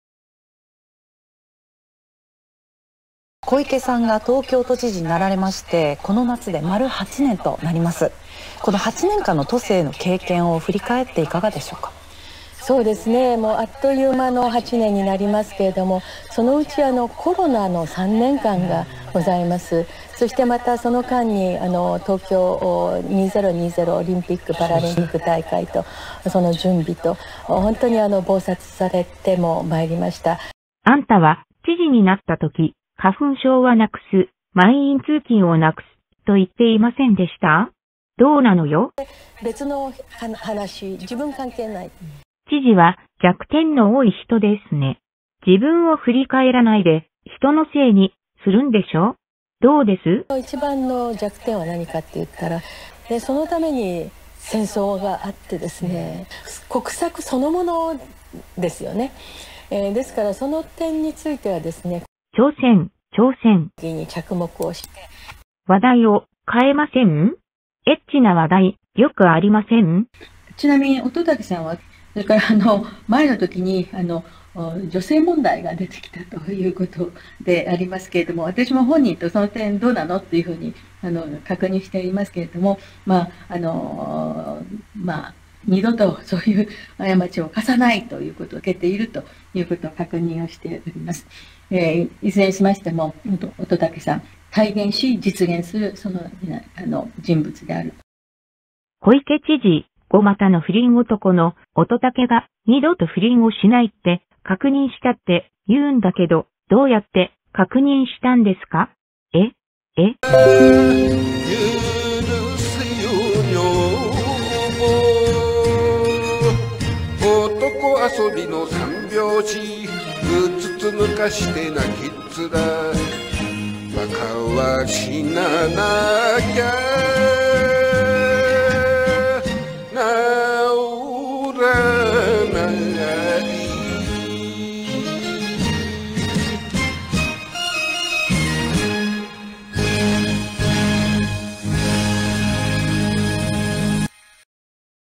小池百合老婆,豚うんこ悪臭が消えない,汚いのは性格と小池都政だ。乙武の不倫再発止める性犯罪システム導入か。
引用：テレ東,東京都知事　小池 百合子（2024年4月7日。